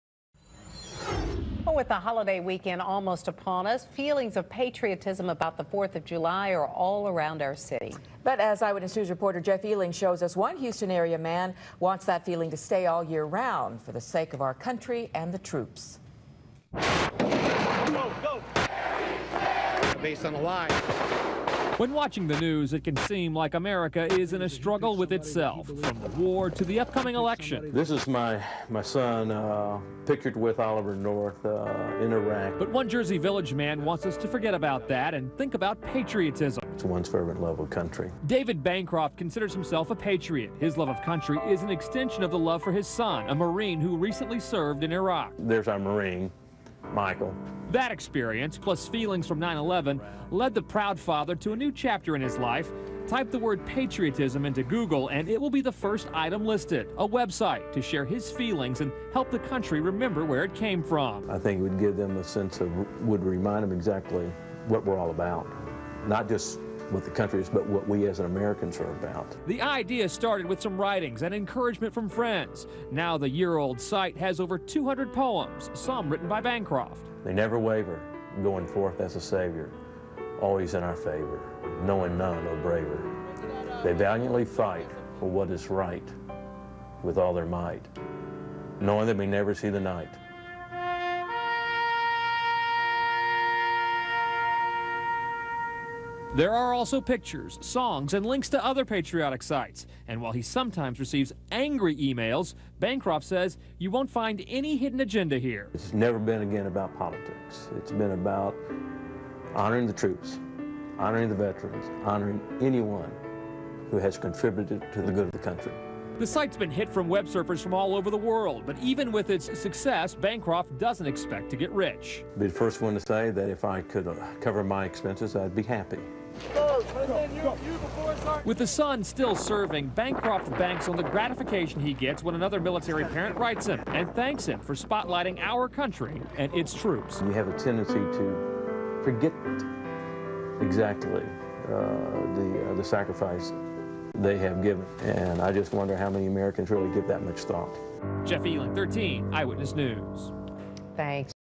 Interview of David Bancroft about founding USA Patriotism!
David Bancroft, USA Patriotism! founder, talks about the reason and purpose of USA Patriotism! including his youngest son's combat duty in Iraq during 2003 with the Marines. (Filmed on June 30, 2004 by Houston ABC affiliate)

USA Patriotism!